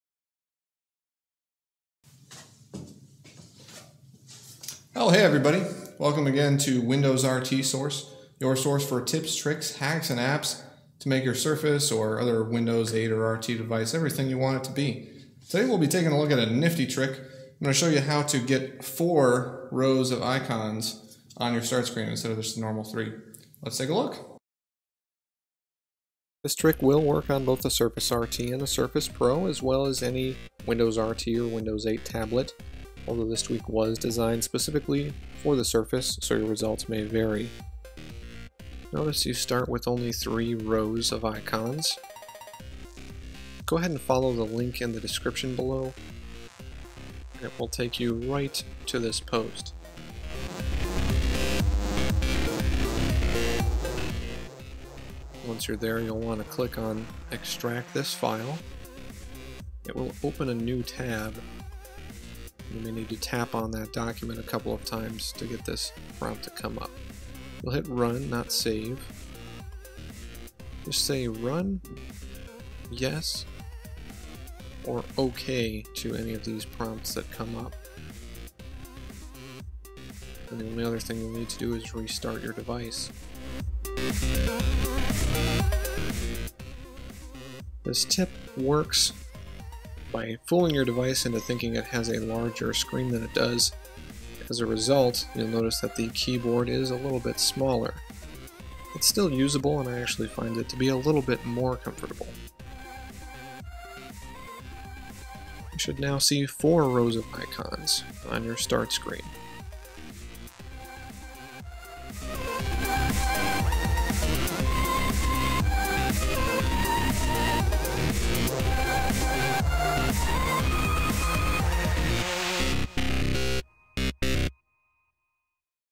Microsoft Surface Hack | 4 Live Tile Rows On Start Screen
- We normally cover the best apps for Surface RT and Surface Pro, but we're ultimately interested in making your Microsoft Surface the best it can be. This video shows you how to install a Windows Registry Hack that will allow you to increase the rows of Live Tiles you can have on your Windows 8 Start Screen.